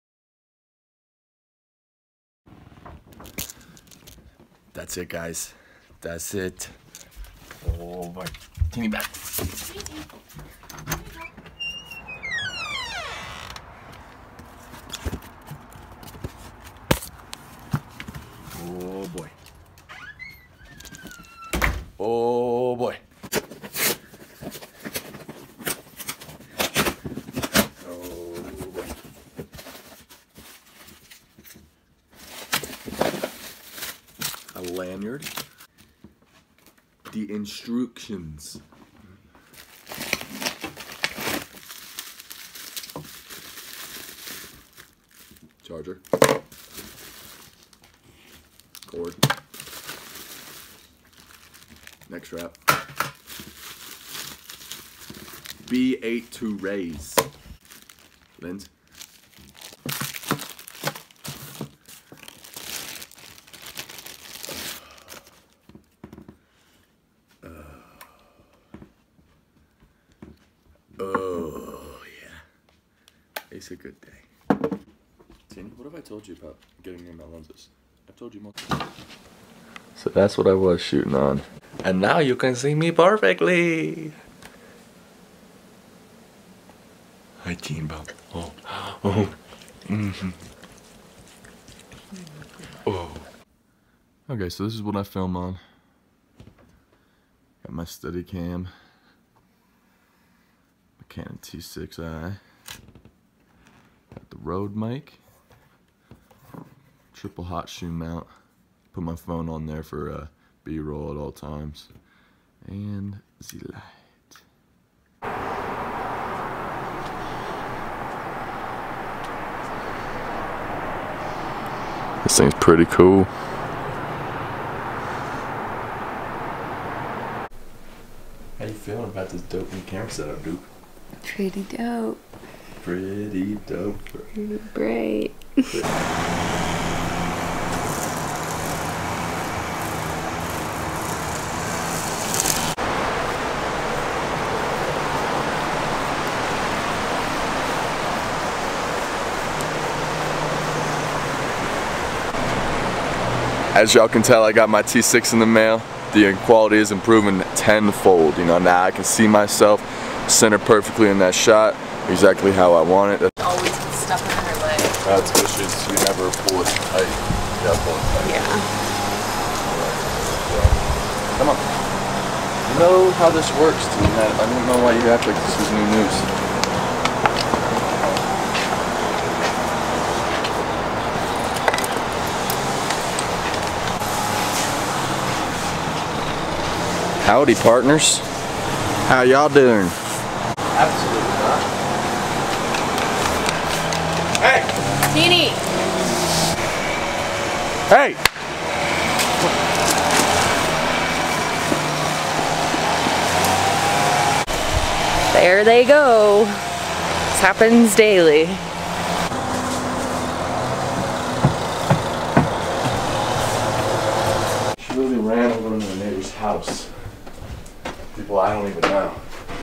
Vlog_003 New Gear Like It’s Christmas
So, after only a few long days of waiting, I finally received my camera in the mail. Canon t6i and it makes everything about this experience much more precise.
Thanks for all continued support. Looking forward to producing better content now for you guys.